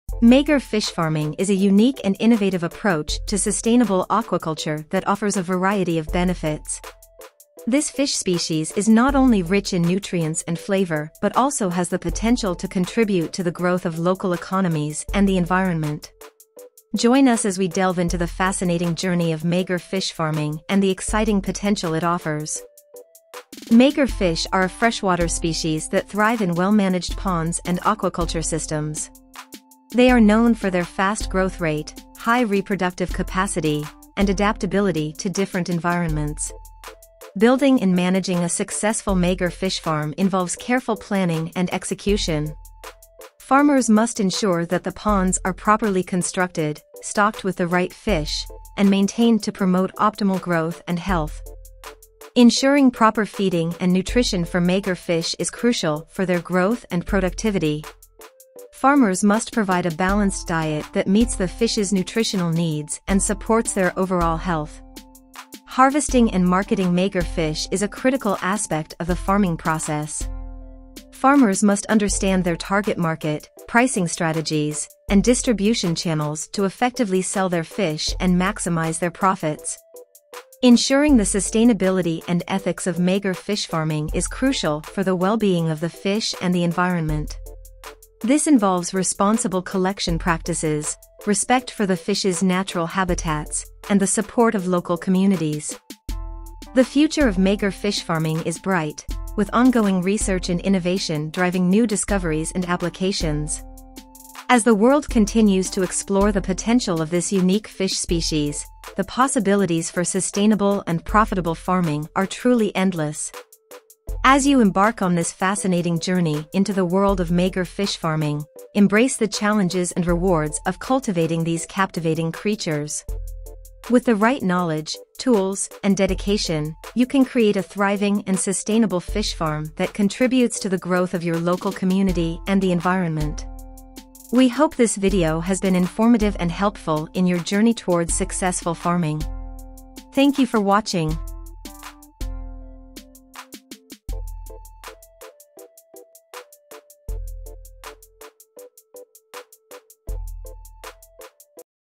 Magur (Catfish) Fish Farming - Complete Guide
Welcome to our ultimate Magur (Catfish) Fish Farming - Complete Guide! Dive into the fascinating world of catfish farming and learn everything you need to successfully rear this remarkable species. In this detailed video, we’ll cover essential tips and tricks—from setting up your fish farm, optimizing feeding methods, to ensuring the perfect water conditions for Magur fish.

Whether you're a seasoned aquaculturist or a beginner exploring fish farming, our guide is tailored for YOU! Learn profitable techniques, avoid common pitfalls, and discover market insights for Java catfish that will ensure a bountiful harvest.

0:00 Magur Fish Farming
0:23 Magur Fish Biology
0:36 Pond Construction & Management
0:51 Fish Feeding & Nutrition
1:05 Harvesting & Marketing
1:20 Sustainability & Ethics
1:36 The Future of Magur Fish Farming
1:53 Conclusion